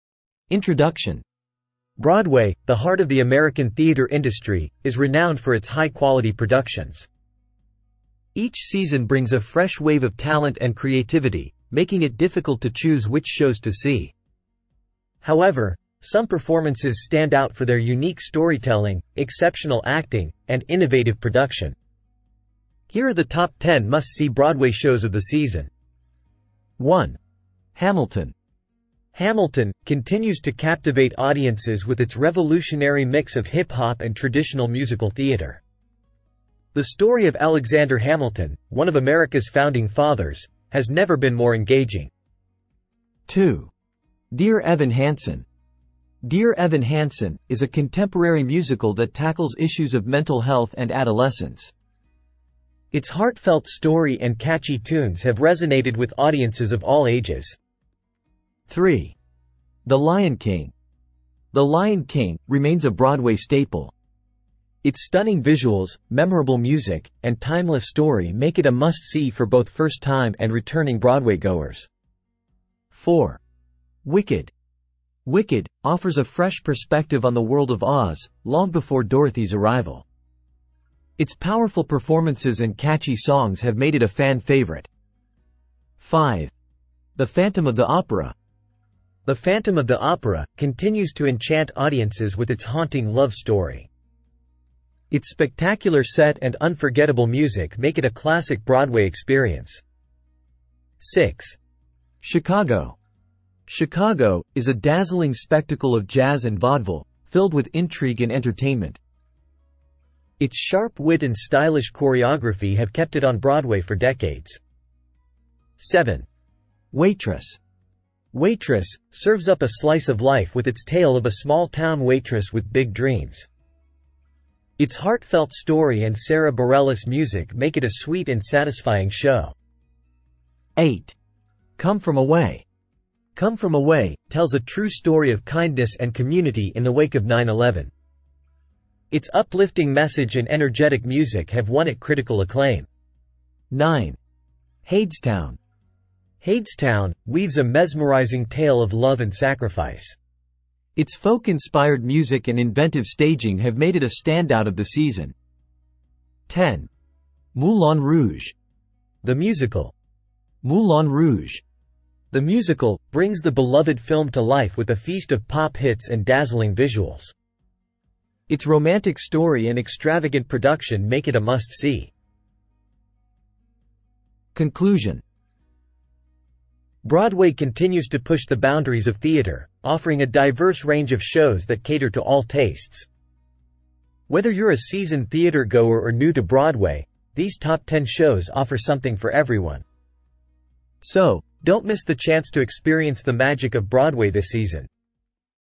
Top 10 Must See Broadway Shows of the Season
Experience the magic of Broadway with our list of the top 10 must-see shows of the season. From dazzling musicals to captivating dramas, these productions promise unforgettable performances and theatrical excellence.